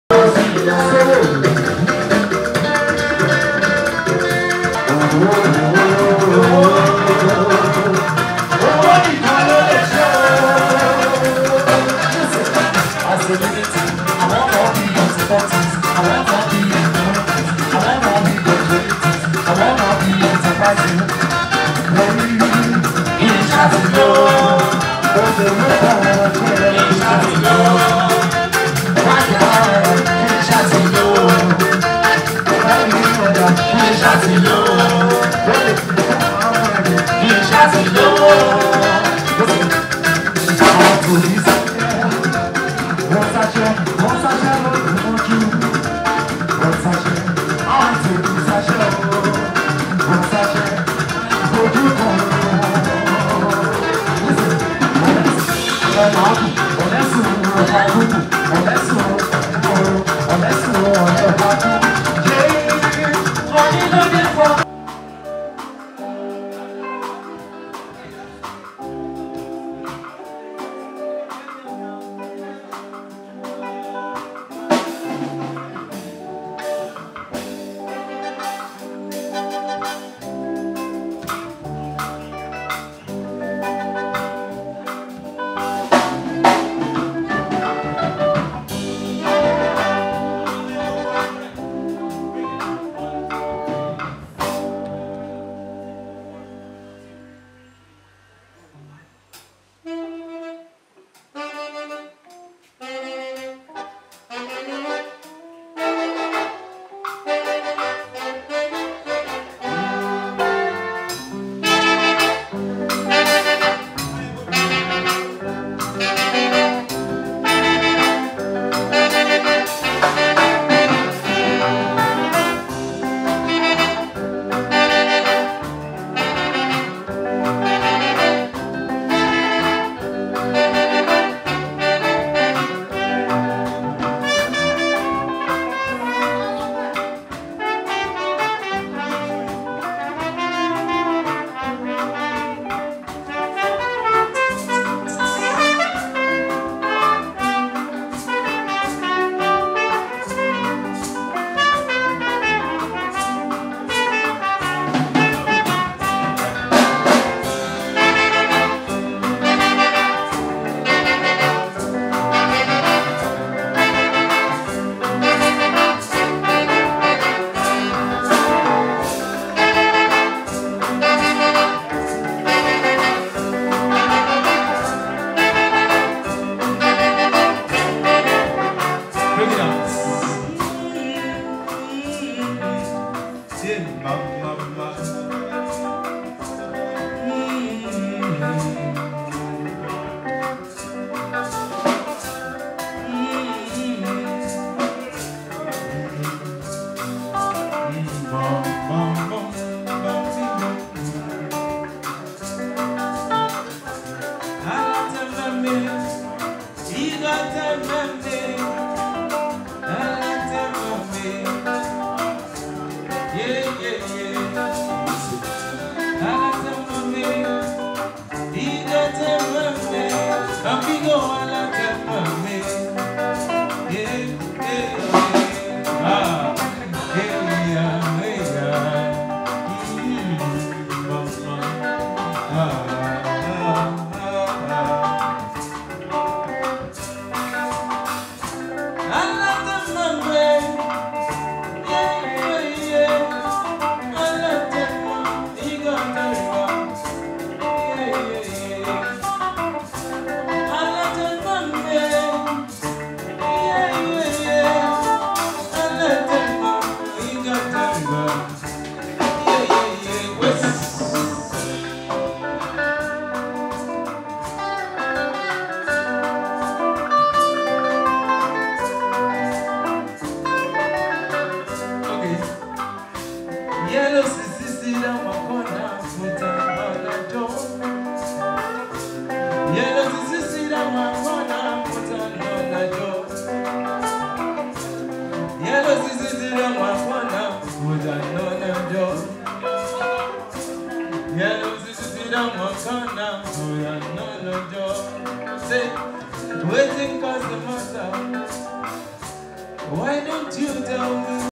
Lavolta Stars at Prest Jazz Club, Mar 01 '18
Lavolta Stars Performing at the Prest Jazz Club, Lekki Phase 1 Mar 01 2018.